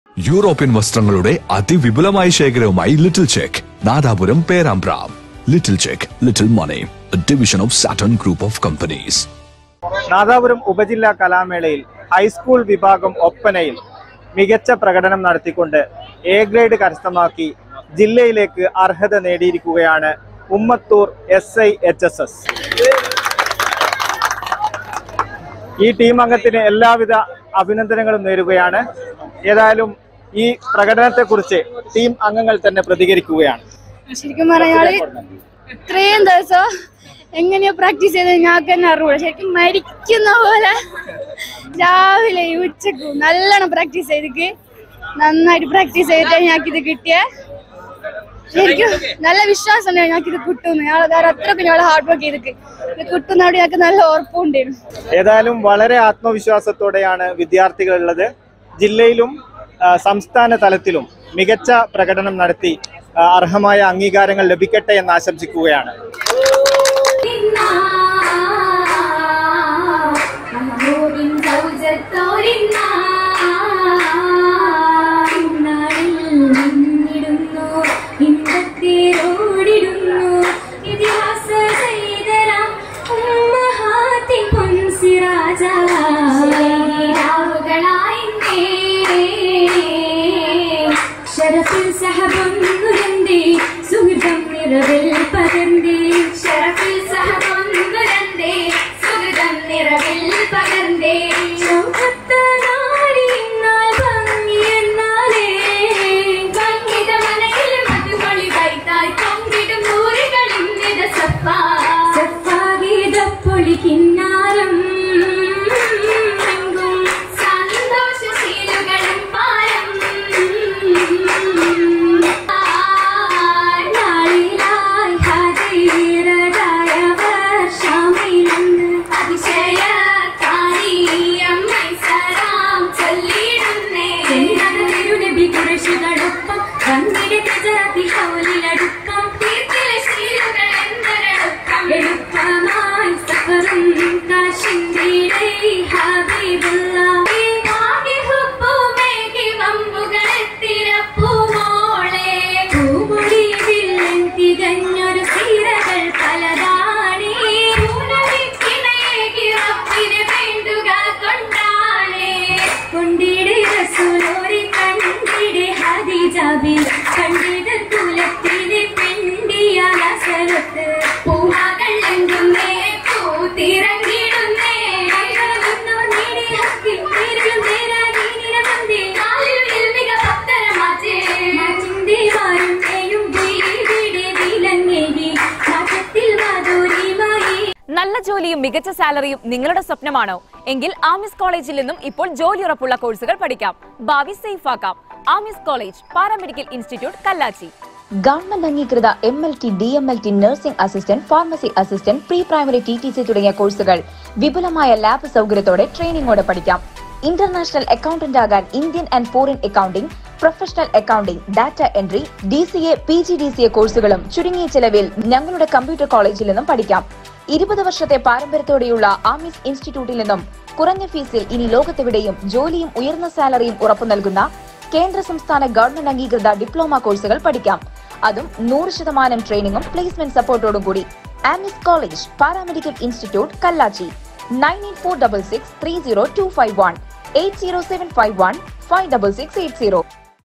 സദസ്സിന്റെ മനം കവർന്ന് മണവാട്ടിയും തോഴിമാരും; ഹൈസ്കൂൾ ഒപ്പനയിൽ മിന്നും പ്രകടനവുമായി ഉമ്മത്തൂർ സ്കൂൾ
Media Vision is the leading online news portal in Vatakara Taluk.